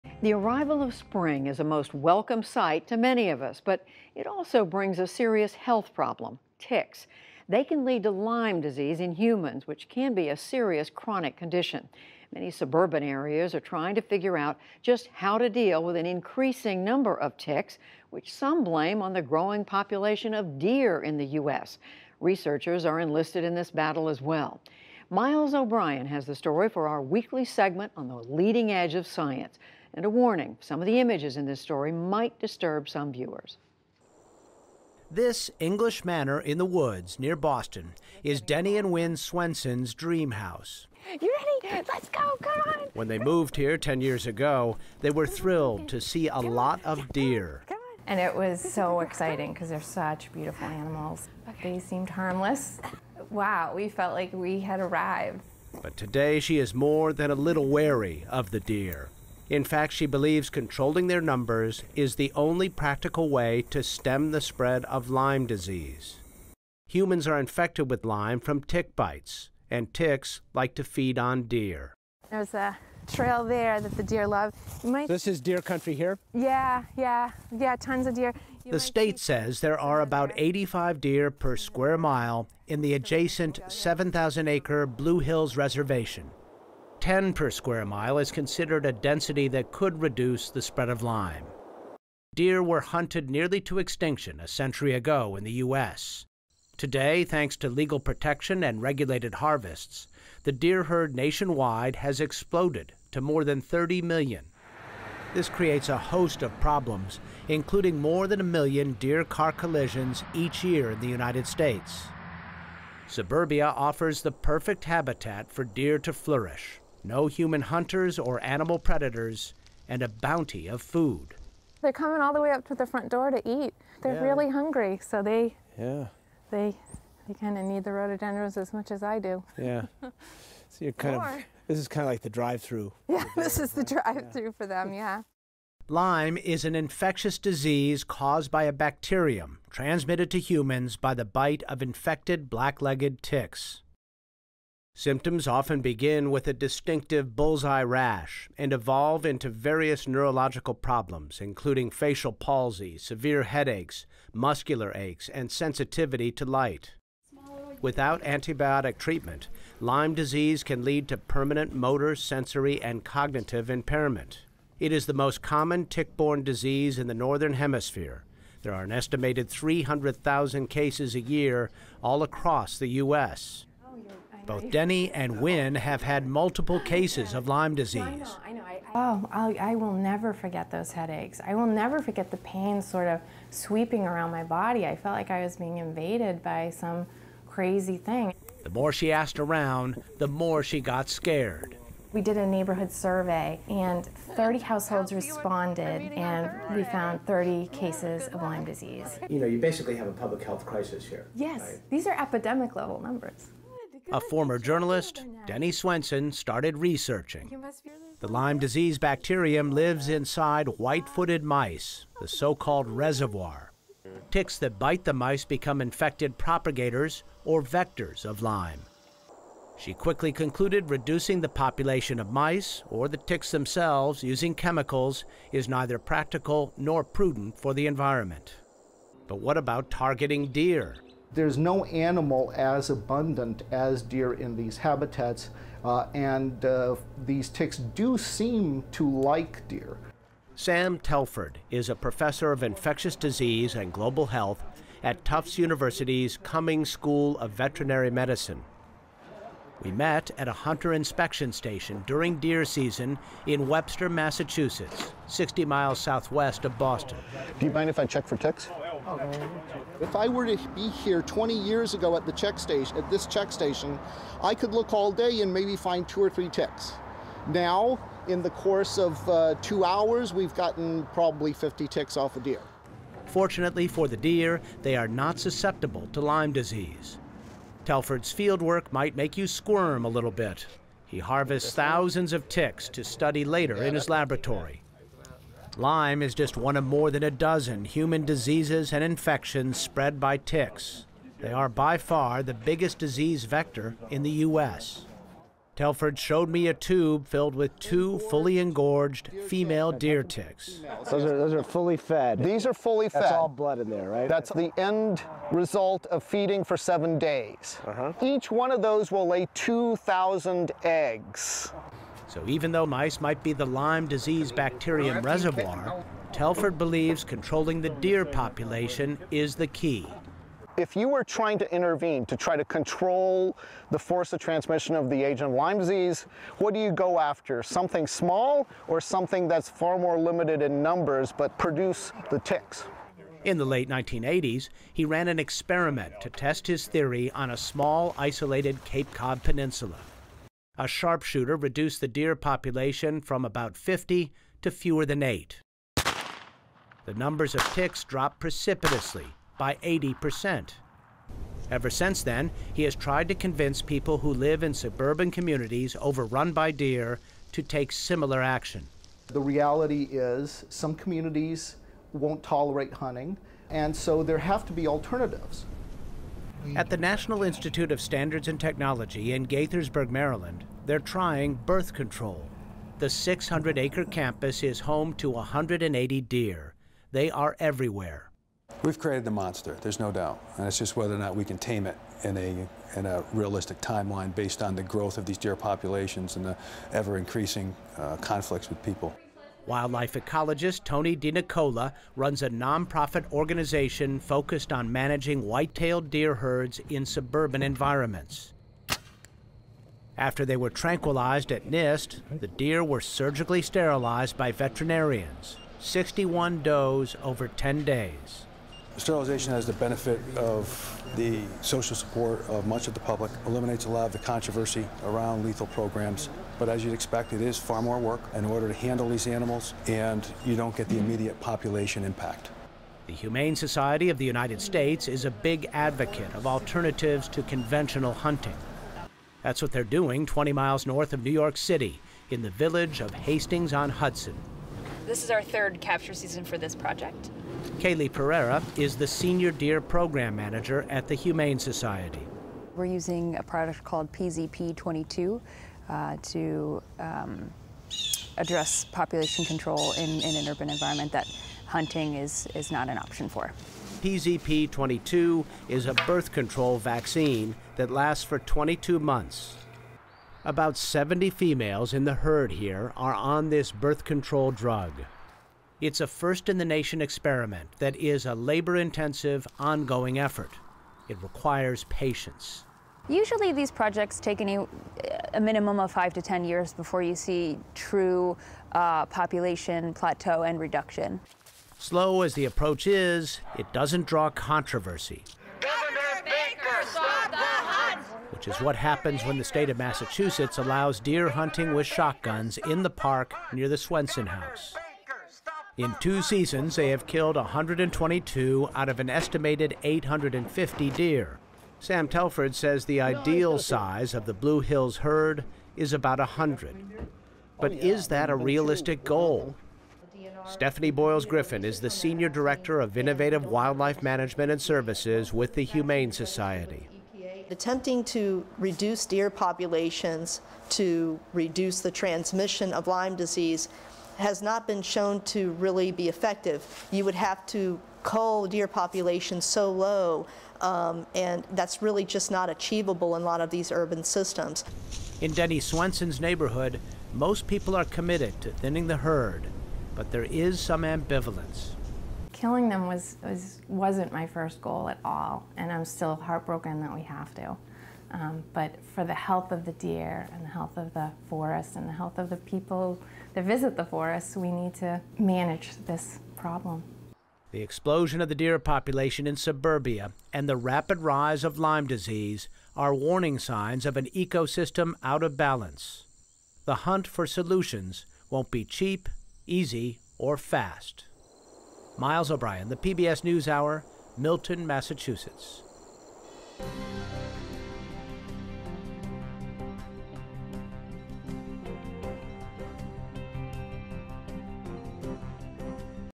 Why finding a solution to control Lyme disease isn’t simple
Ticks are by far the biggest disease vector in the U.S., and Lyme disease is the most common tick-borne illness in the northern hemisphere, causing neurological problems and even permanent cognitive and sensory impairment if not treated. Science correspondent Miles O’Brien reports on how communities are trying to combat the spread of the disease by controlling through hunting and alternatives.